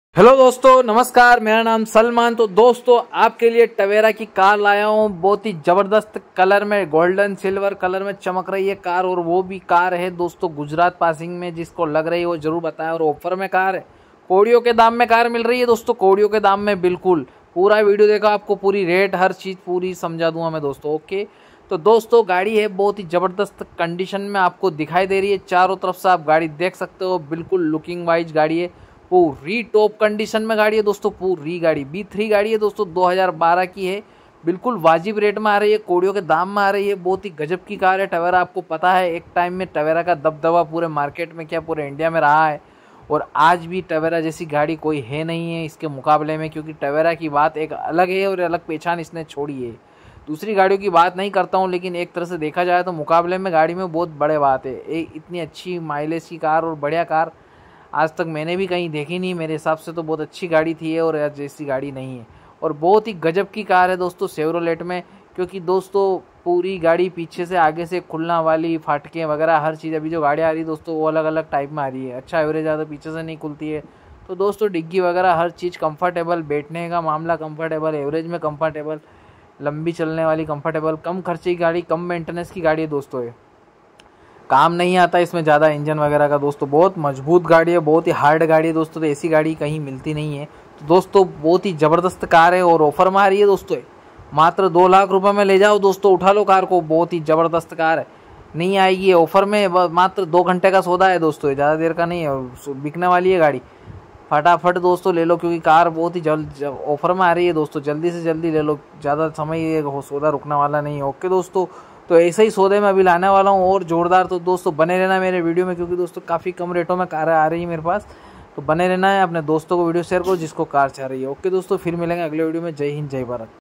TAVERA DIESEL 2012 || SALE FOR CAR CHEVEROLET || LOAN PER CAR TAVERA || टवेरा जल्दी बेचना है
,,,  हेलो दोस्तों इस वीडियो में मैंने टवेरा कार को बेचना है पूरी जानकारी दी है देख सकते हो,,,